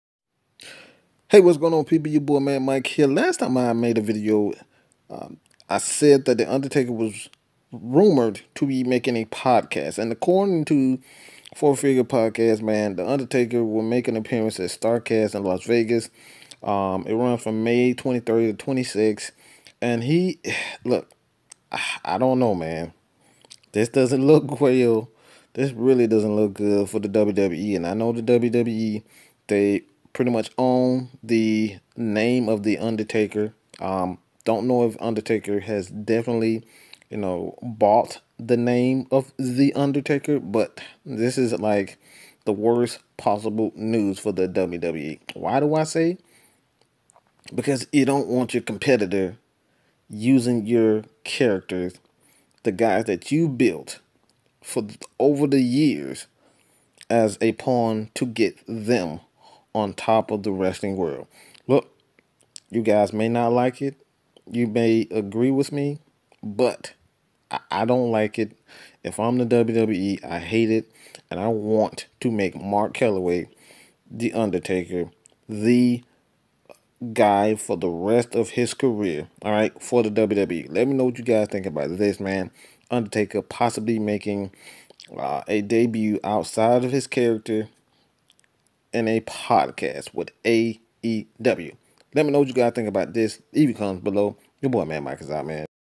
Undertaker News | Another Date Confirmed By Figure Four Podcast
Undertaker News coming at you Wrestling Fanatics. This time it's an update on the latest news surrounding the legend himself, Mean Mark Callaway.